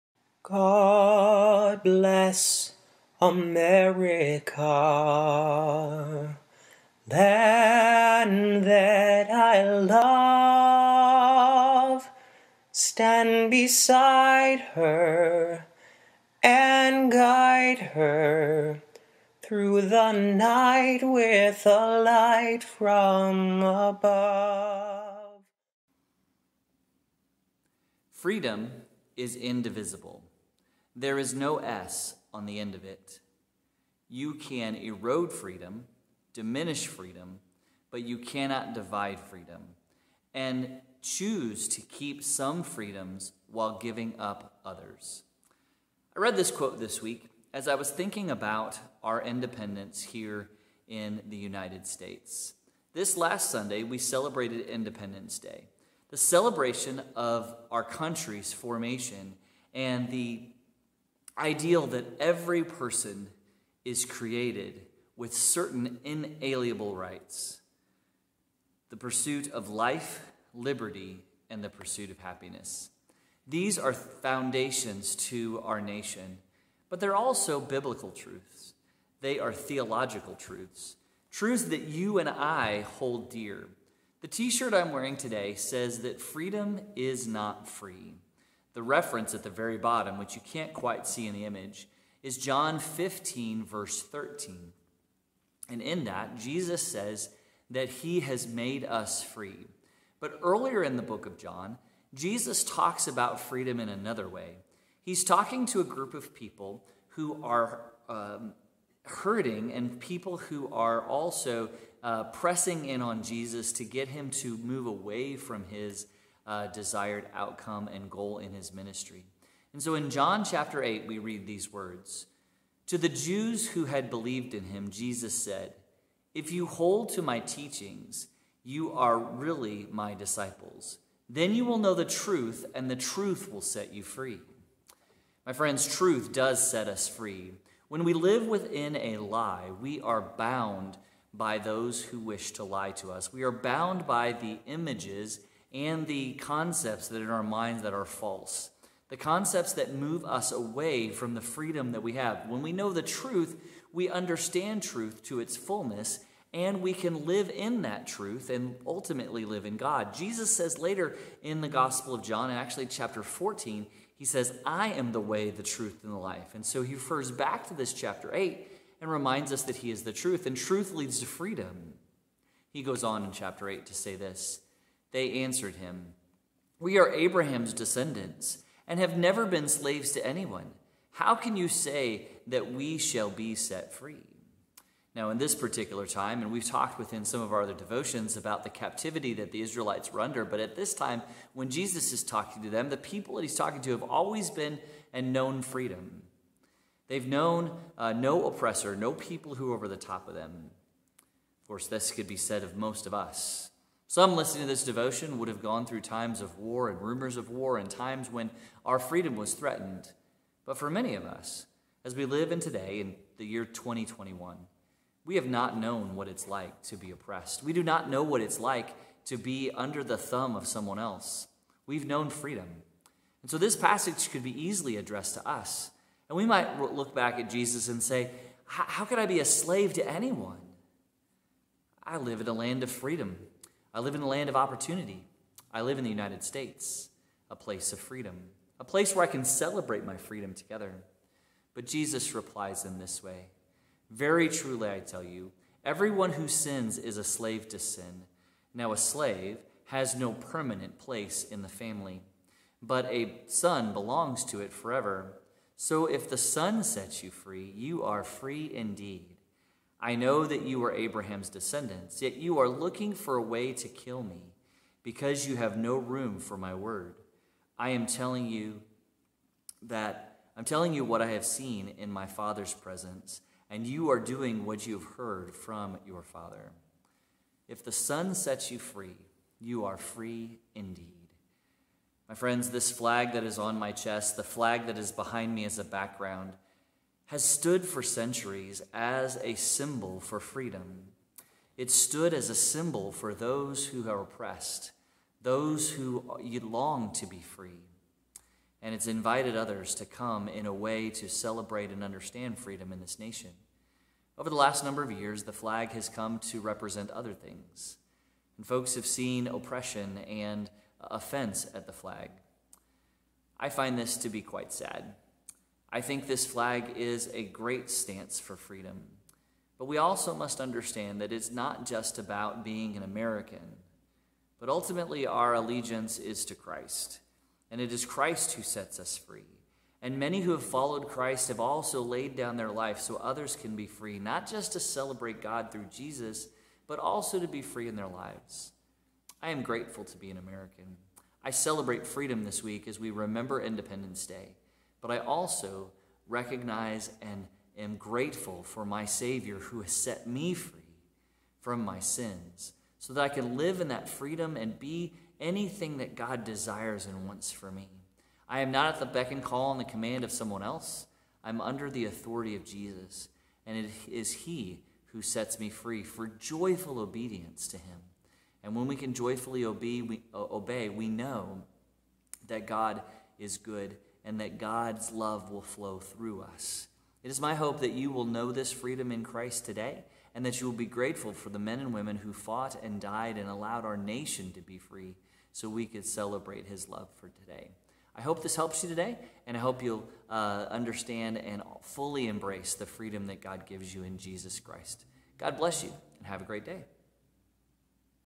Devo 7 7 21 Celebrate Freedom
Happy Independence Week!

Intro Song by Patrick Jennings 5 (downloaded from Soundcloud)